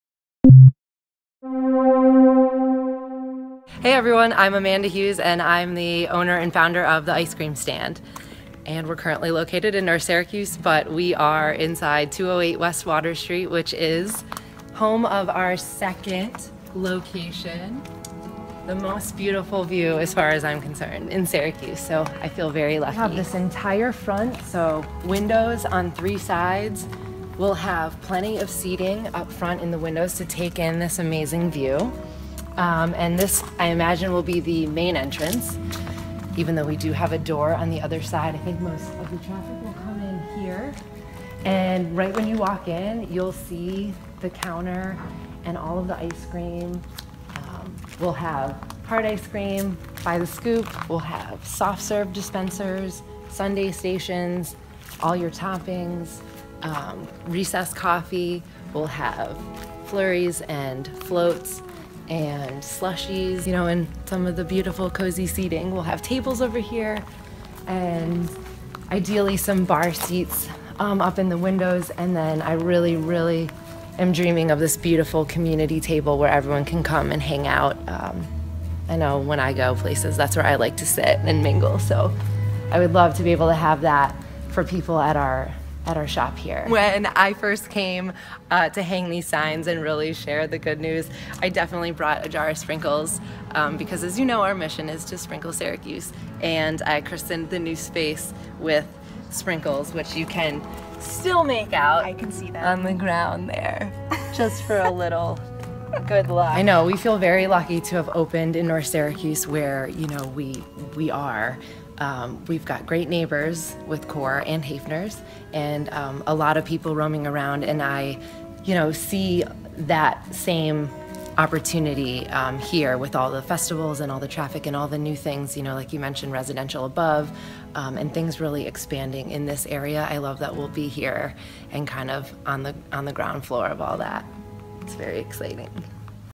The Ice Cream Stand to open Clinton Square location
Amanda Hughes shows reporter Katrina Tulloch around the future home of The Ice Cream Stand in downtown Syracuse.